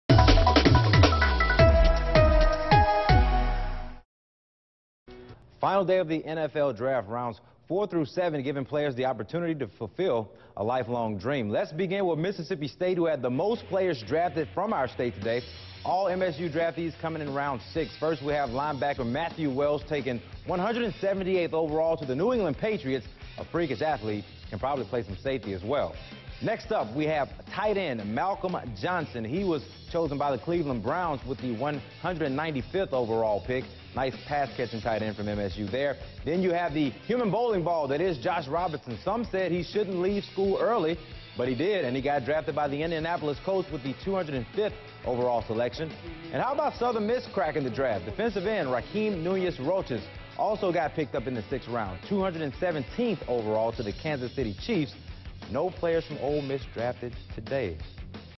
Mississippi shines in final day of NFL Draft
Three Mississippi State players and one Southern Miss player were chosen on the final day of the NFL draft.